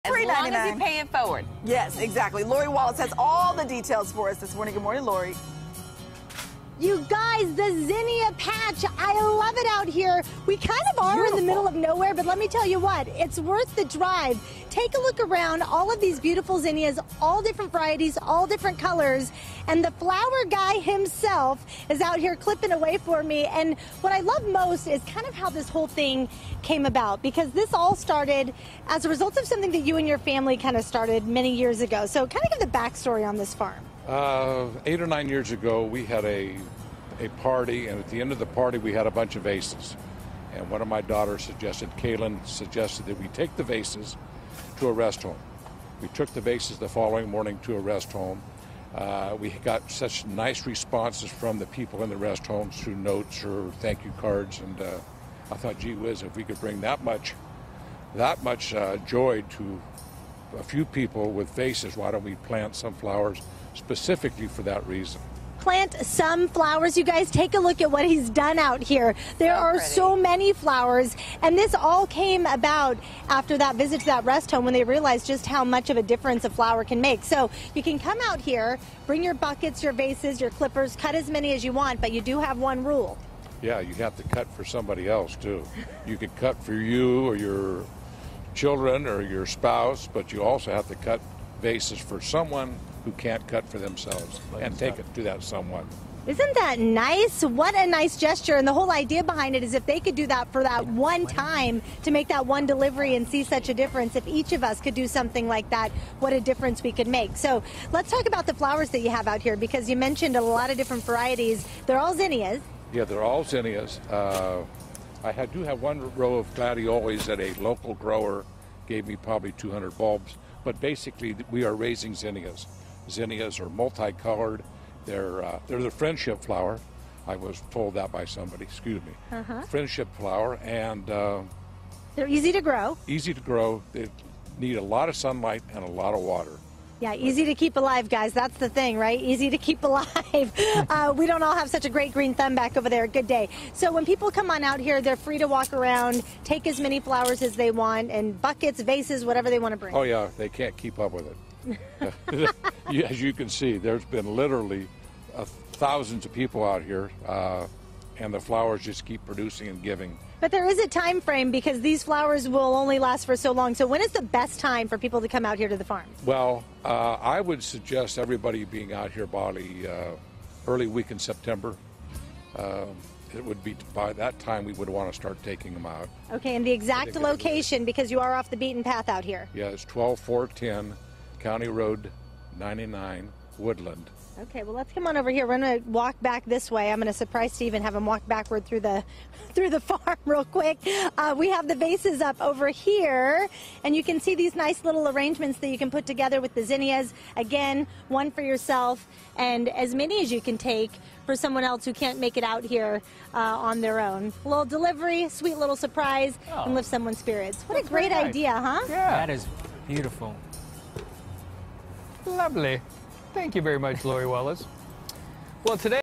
Friendship Flowers
Lori Wallace has the scoop on these flowers that help you pay it forward.